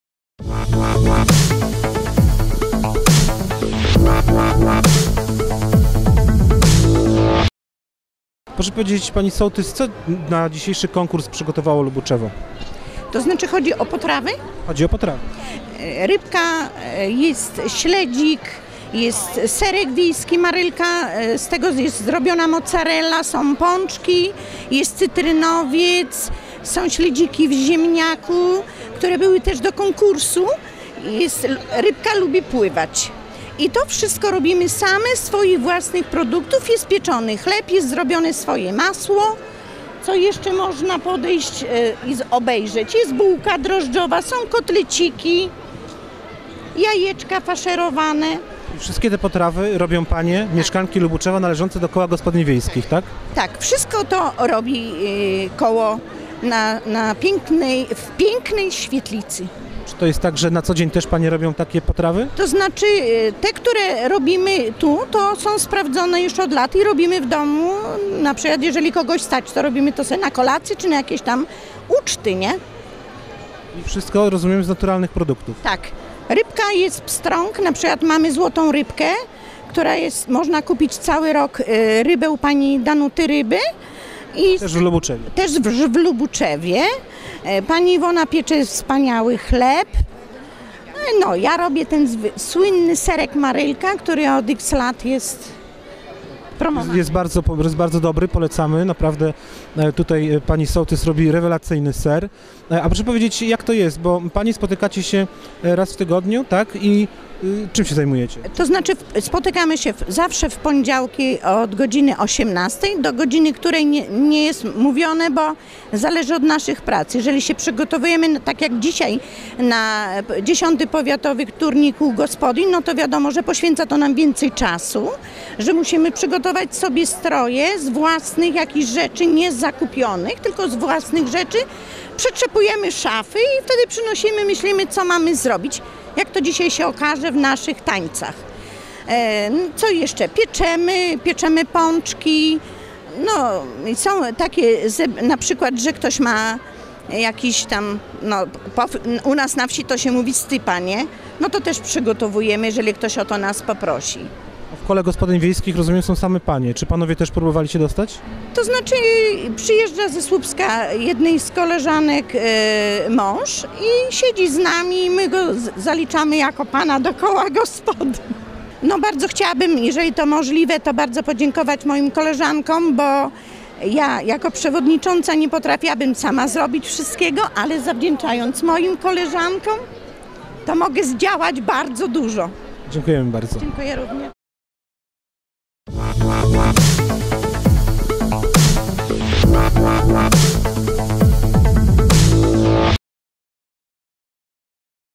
GRYF24.pl | 22.02 | X Powiatowy Turniej KGW - Rozmowa z sołtys Lubuczewa
GRYF24.pl | 22.02 | X Powiatowy Turniej KGW - Rozmowa z sołtysem Lubuczewa